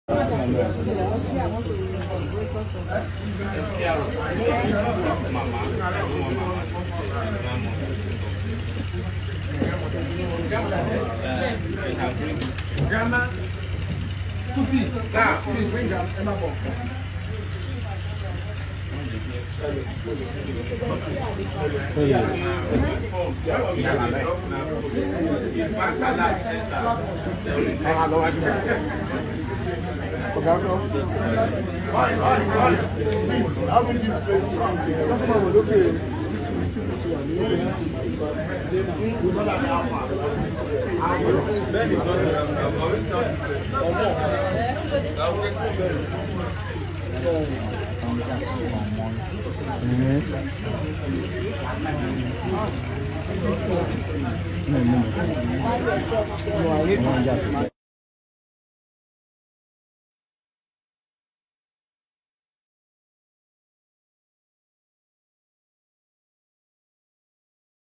Lagos Police invite Mohbad Daddy for more questioning.
we bring you new video of Mohbad, police officer invite Mohbad Daddy for questioning on Mohbad déath.